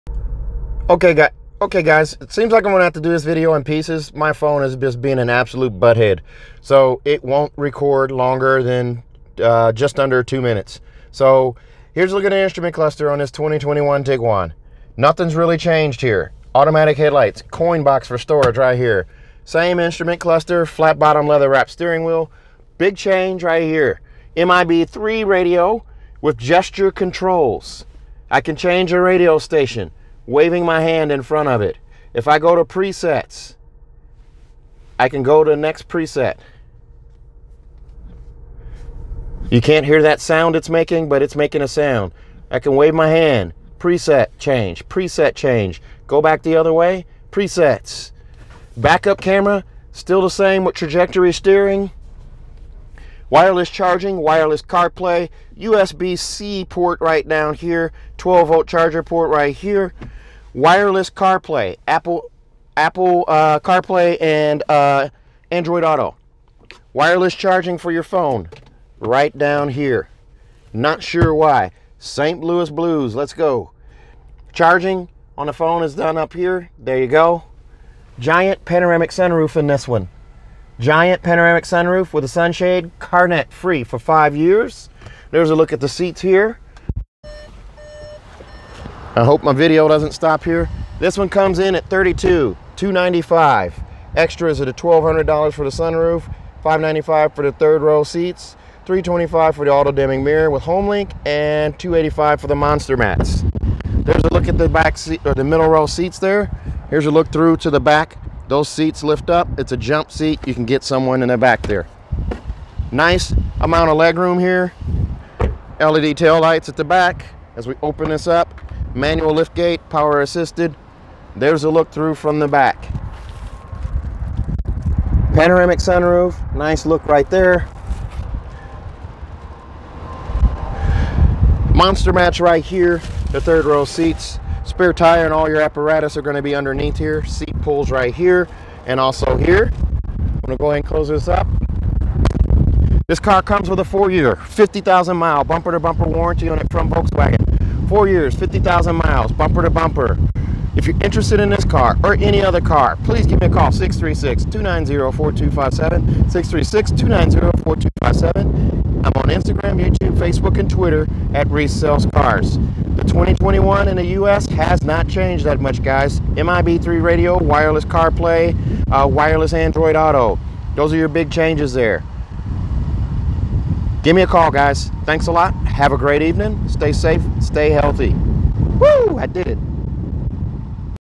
2021 VW Tiguan 2.0T SE 4Motion B Roll with technical difficulties
2021 VW Tiguan 2.0T SE 4Motion B Roll with technical difficulties

Platinum Gray Metallic exterior/Black leatherette